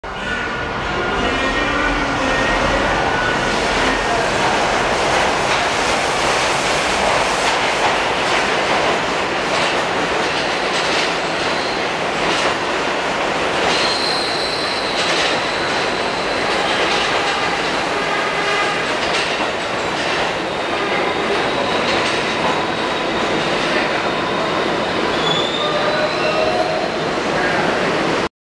R142A 6 train entering Astor Place
Downtown 6 train coming into Astor Place,